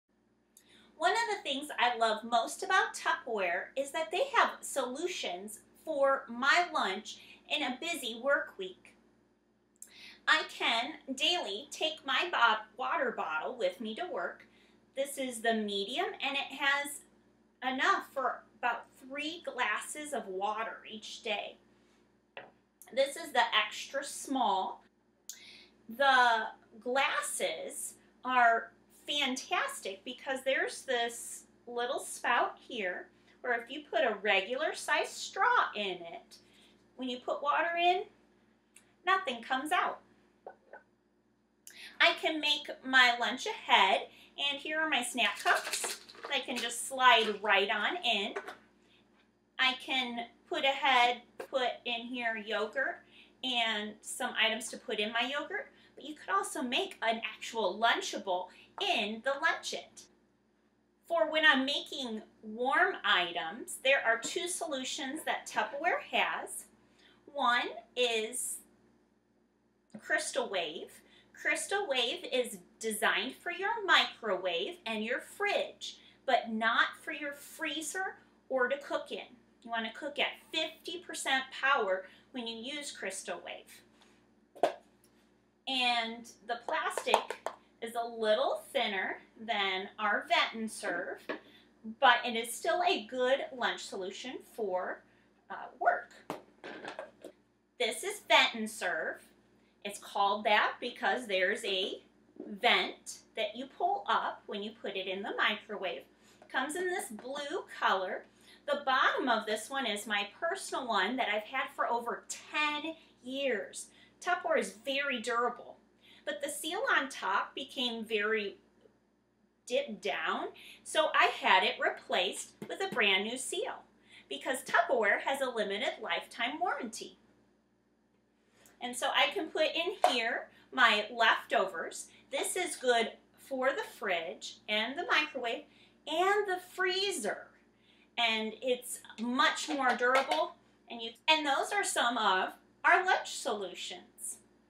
Lunch Solutions
This video is about Lunch Solutions that Tupperware makes.